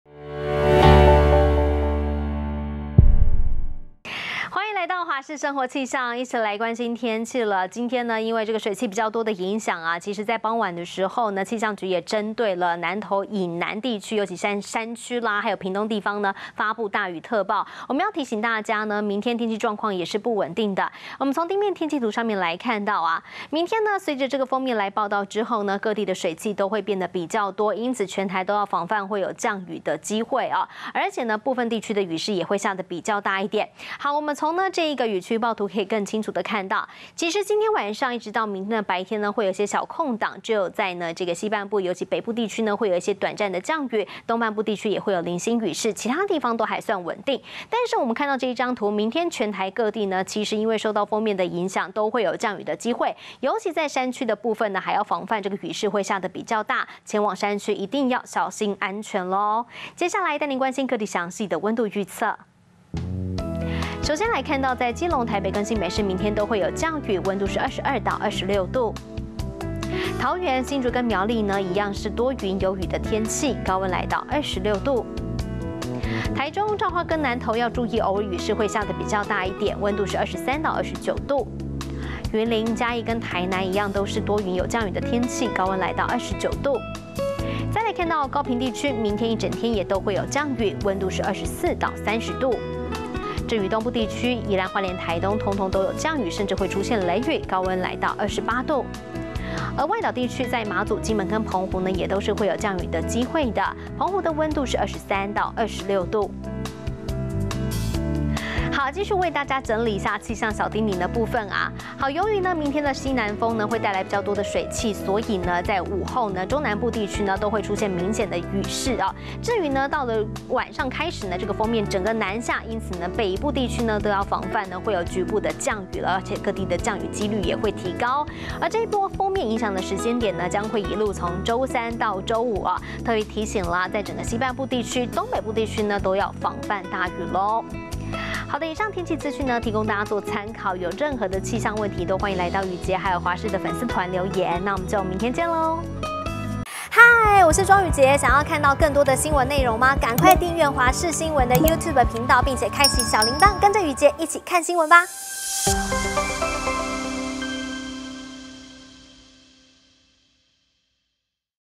今晚起鋒面逼近 各地降雨機率增｜華視生活氣象｜華視新聞 20220523
今(23)日鋒面位在臺灣北部海面，各地雲量偏多，北部、南部有短暫陣雨發生，尤其南部山區雨勢比較大，高雄山區達到短延時大雨標準；北臺灣最高溫約25到27度，中臺灣以南最高溫約28到31度。